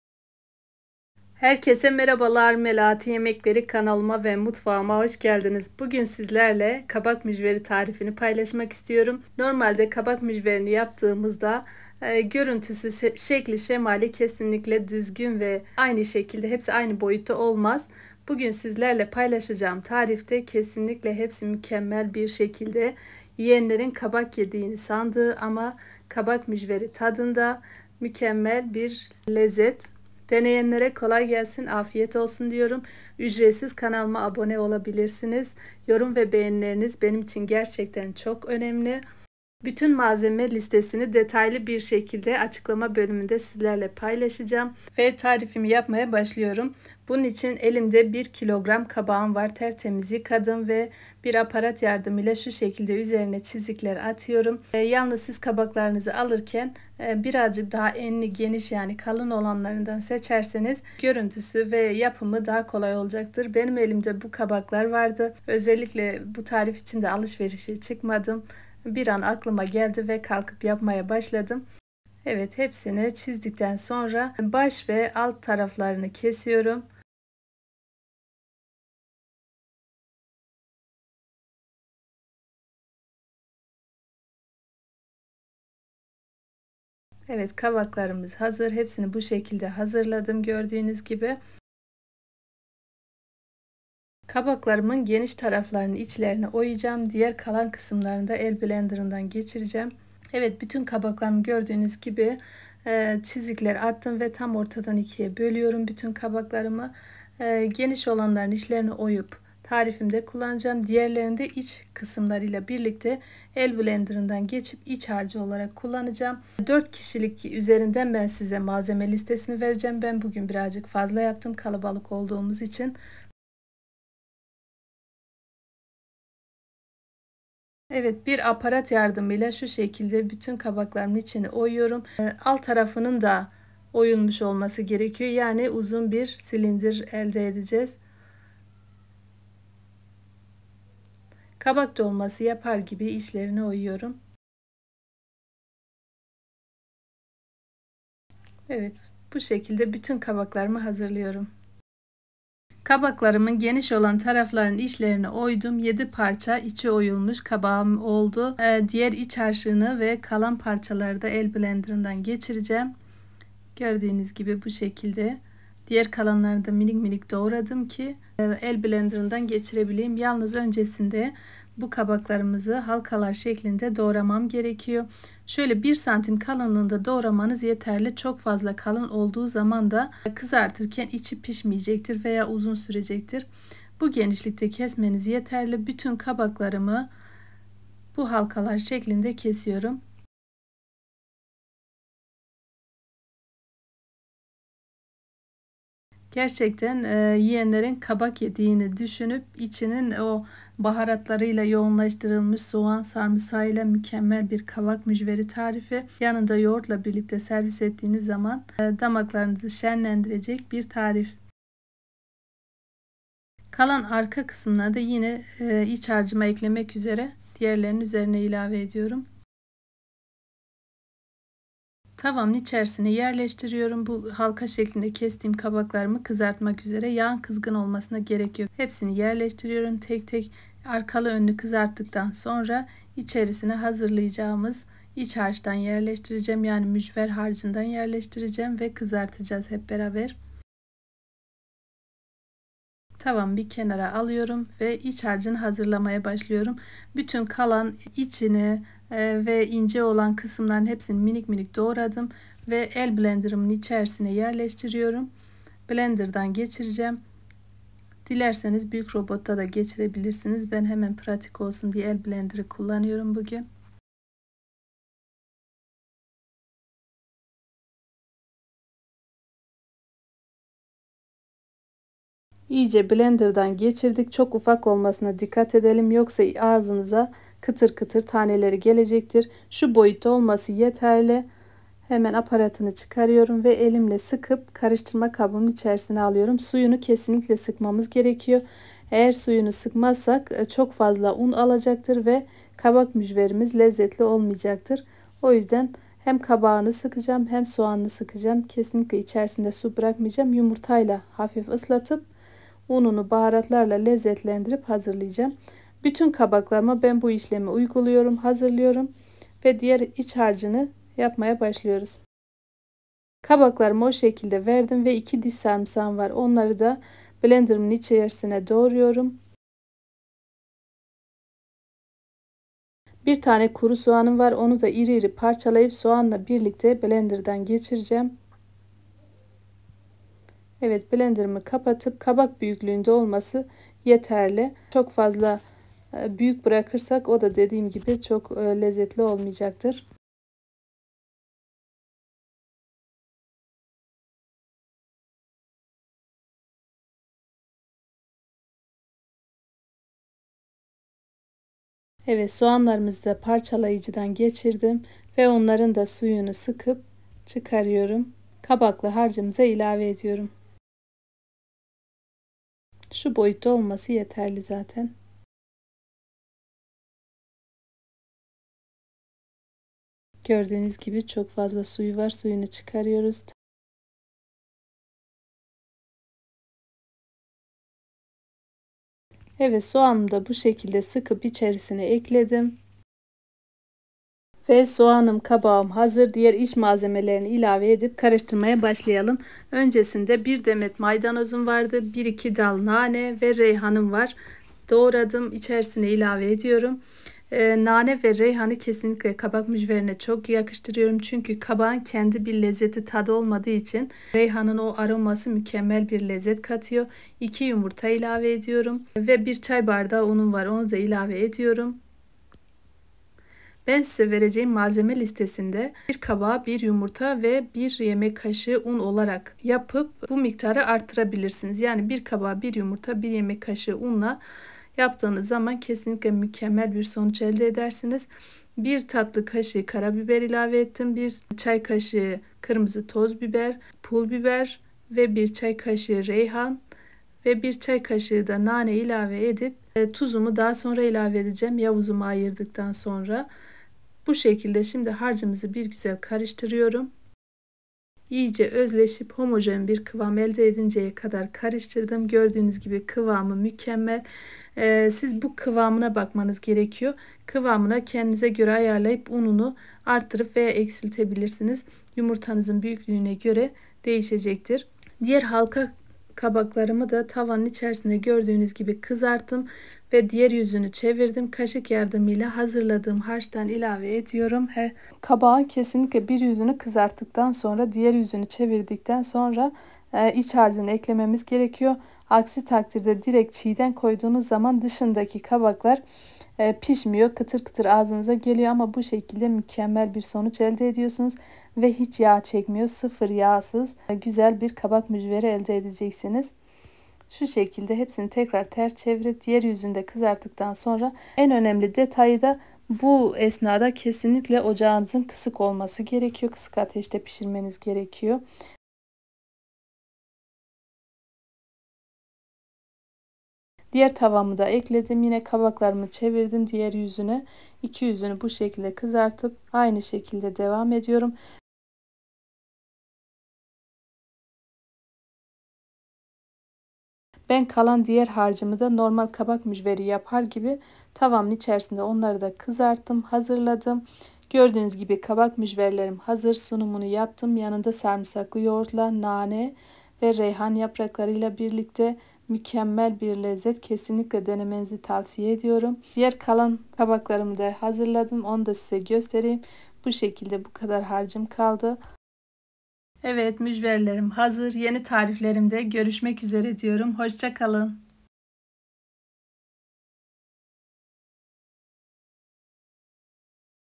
KABAK MÜCVERİ TARİFİ | NASIL YAPILIR??👍
Şekli şahane lezzeti mükemmel kabak mücveri tarifi çok kolay nefis pratik tarif için

Malzemeler:
2 Tane orta boy kabak
2 Tane yumurta 🥚
1 Çay bardağı un
1 Tane kuru soğan 🧅
Yarım Demet maydanoz 🌿
2 Dal nane
1 Dal Reyhan
2 Diş sarımsak 🧄
1 Tatlı Kaşığı tuz 🧂
1 Çay kaşığı karabiber
1 Çay kaşığı kırmızı toz biber
1 Çay kaşığı pul biber
1 Çay kaşığı Reyhan
1 Çay kaşığı nane

Deneyenlere kolay gelsin afiyet olsun 😋

Its shape is amazing, its taste is perfect, the recipe for pumpkin muffins is very easy and delicious, for a practical recipe

Materials:
2 medium zucchini
2 eggs 🥚
1 tea glass of flour
1 onion
Half a bunch of parsley 🌿
2 sprigs of mint
1 Branch of Basil
2 cloves of garlic
1 teaspoon salt
1 teaspoon of black pepper
1 teaspoon red chili powder
1 teaspoon paprika
1 teaspoon of Reyhan
1 teaspoon mint

Good luck to those who try it 😋

Forması heyrətləndiricidir, dadı mükəmməldir, balqabaqlı kekler üçün resept çox asan və dadlıdır, praktik bir resept üçün

Materiallar:
2 ədəd orta balqabaq
2 yumurta
1 çay stəkanı un
1 soğan
Yarım dəstə cəfəri
2 budaq nanə
1 Fesleğen budağı
2 diş sarımsaq
1 çay qaşığı duz
1 çay qaşığı qara bibər
1 çay qaşığı qırmızı bibər tozu
1 çay qaşığı bibər
1 çay qaşığı Reyhan
1 çay qaşığı nanə

Qızartmaq üçün yağ

Sınayanlara uğurlar 😋
Yeni reseptlərim üçün kanalıma abunə olmağı unutmayın

شكله مذهل ، طعمه مثالي ، وصفة فطائر اليقطين سهلة جدا ولذيذة ، لوصفة عملية

المواد:
2 حبة متوسطة الحجم كوسة
2 بيض 🥚
1 كوب شاي دقيق
1 بصلة
نصف حفنة من البقدونس 🌿
2 غصن نعناع
1 فرع الريحان
2 فص ثوم
1 ملعقة صغيرة ملح
1 ملعقة صغيرة فلفل أسود
1 ملعقة صغيرة من مسحوق الفلفل الأحمر
1 ملعقة صغيرة بابريكا
1 ملعقة صغيرة ريحان
1 ملعقة صغيرة نعناع

زيت للقلي